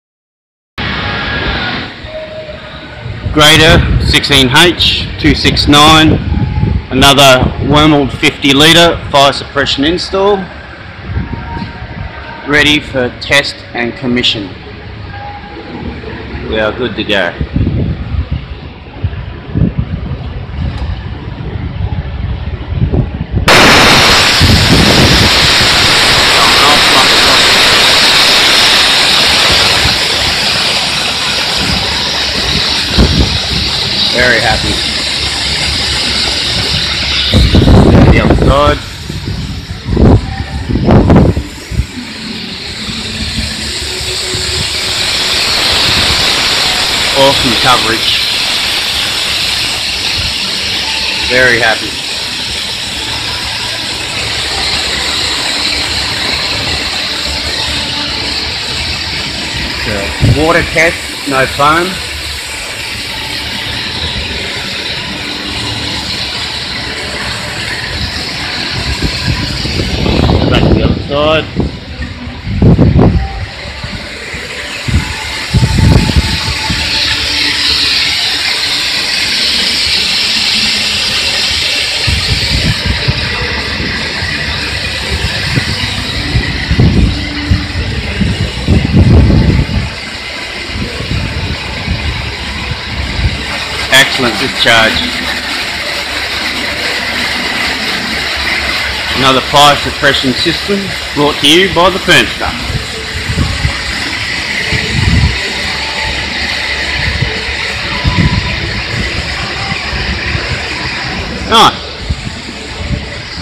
Fire Suppression Install of Wormald LOP AFFF System
Fire Suppression System, AFFF Wormald 50 ltr  Install on Grader 16H, Test and Commission Feb 2017 in Northern Territory Australia.